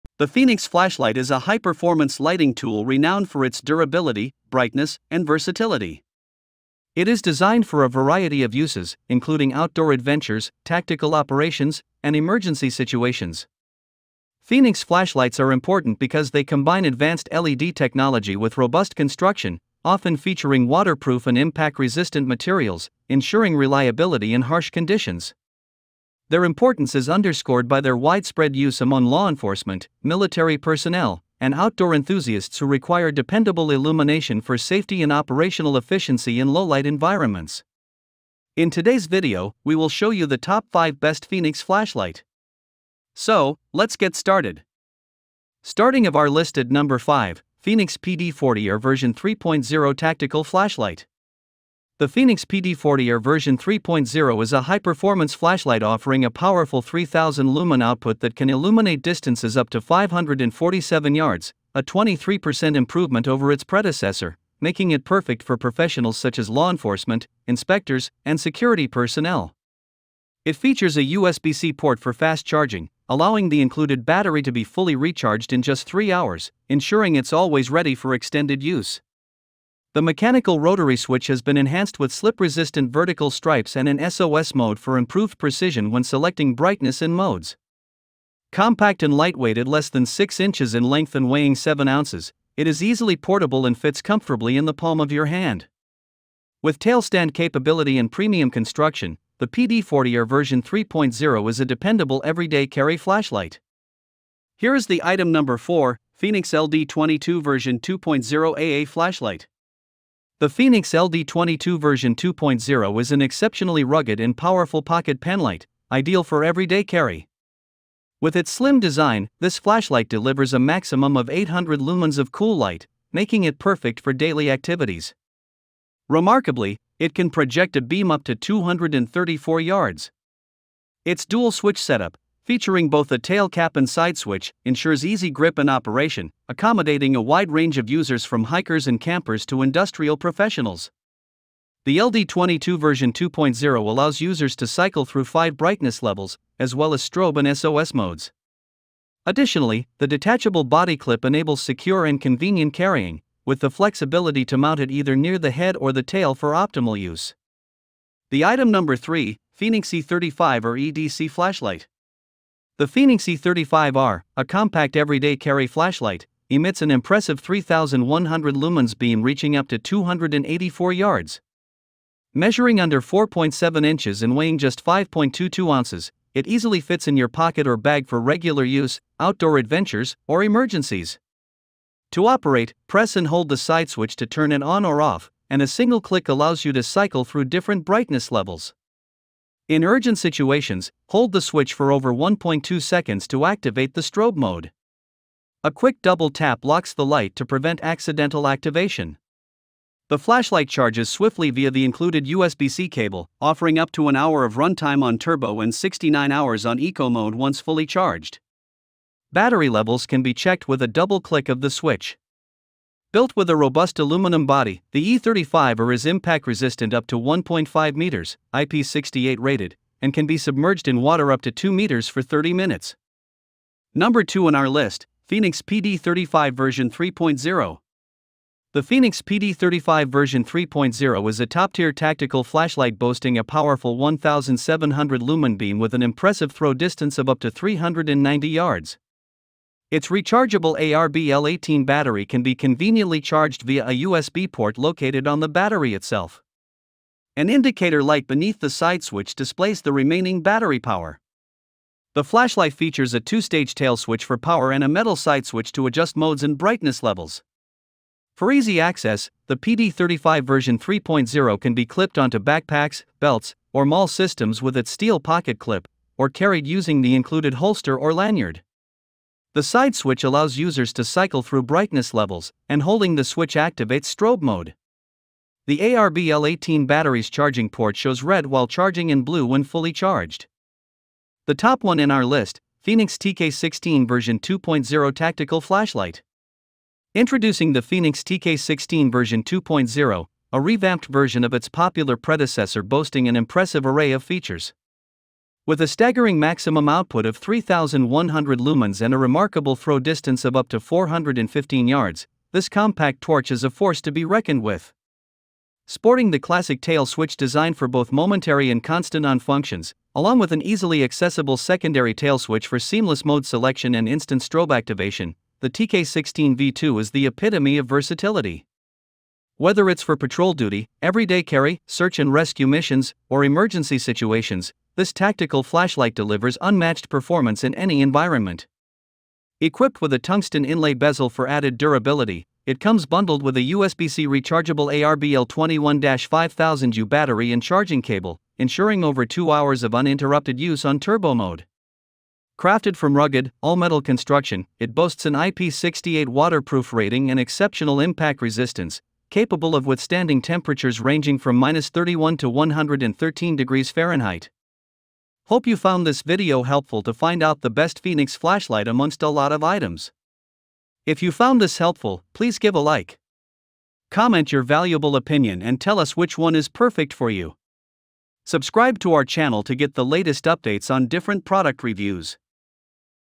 Illuminate Your Night Discover the Power of Fenix Flashlights
5. Fenix PD40R v3.0 Tactical Flashlight

4. Fenix LD22 v2.0 AA Flashlight

3. Fenix E35R EDC Flashlight

2. Fenix PD35 v3.0

1. Fenix TK16 v2.0 Tactical Flashlight

→ Business Mail: @gmail.com